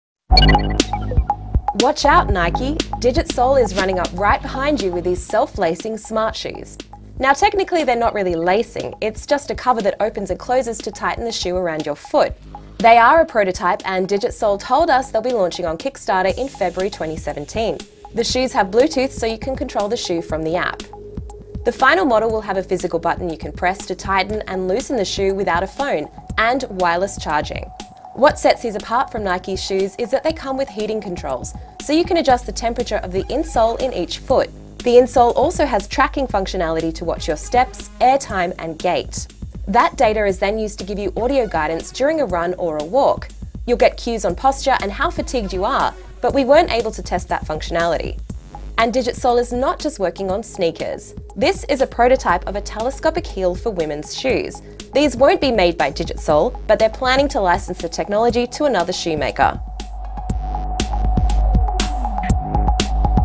나이키도 울고 갈 디짓솔 스마트슈즈 "미래에서 왔나?"
프랑스 벤처기업 디짓솔(digitsole)이 만든
스마트슈즈는 스마트폰 애플리케이션과
연동해 자동으로 신발을 조여주는 미래형 운동화다.

신발에 블루투스가 장착되어 자동으로
신발 커버가 열리고 닫히고 깔창엔 온도조절
장치가 있어 추운 겨울에도 따뜻하게 착용할 수 있다.

여성용 구두도 있는데 이는 블루투스로 스마트폰과
연결해 힐의 높낮이를 조절할 수 있다.

영상에 나오는 신발은 아직 프로토타입이며
2월 중 킥스타터를 통해 완제품을 공개할 예정이다.